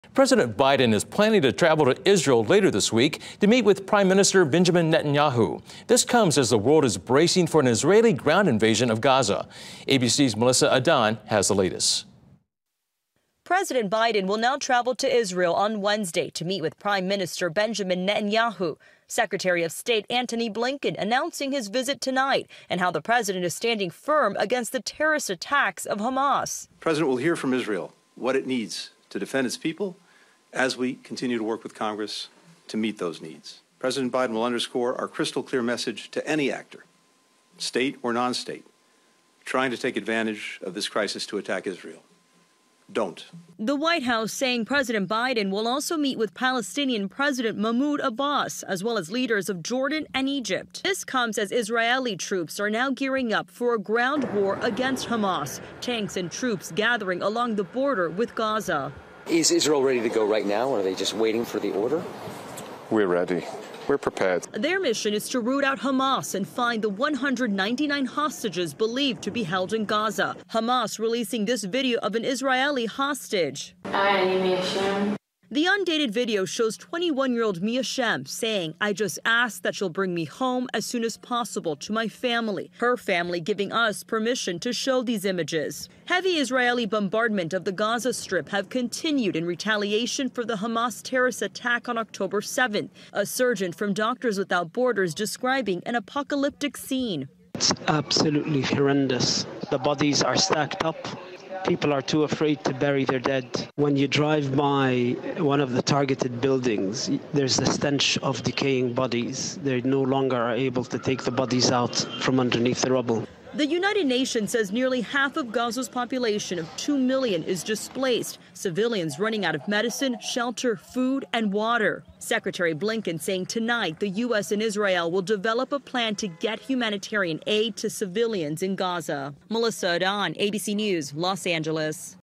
President Biden to visit Israel, as fighting continues and death toll rises
President Biden is planning to travel to Israel later this week to meet with  Prime Minister Benjamin Netanyahu.